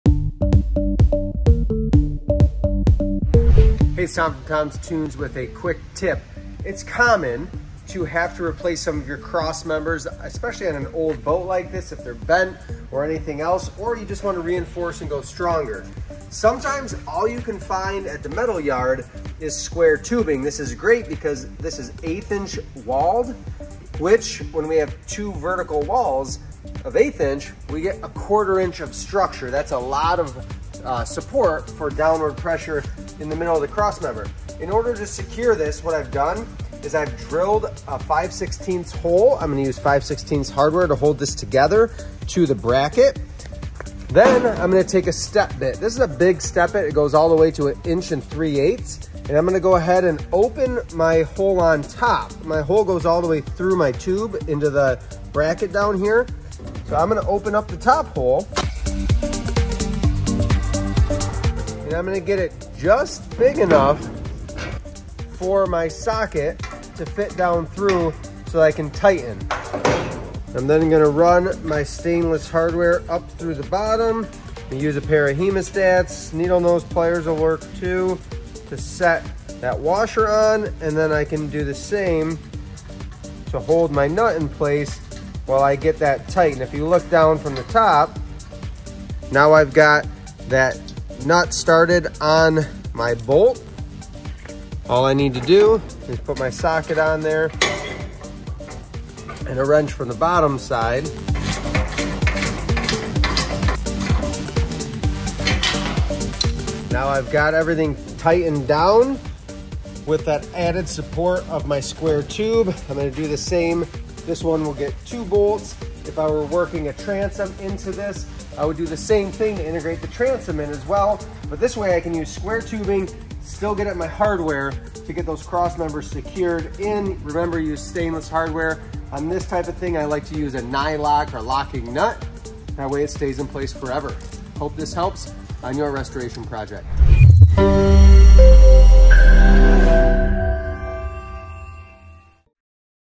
Quick Tip: Installing Square Tube Crossmembers
It's common to only be able to find square tube in the sizes needed to replace or add crossmembers. Or maybe your project requires adding square tube to gain extra support for an outboard upgrade too. Use this tip when installing square or rectangular tube on your pontoon project.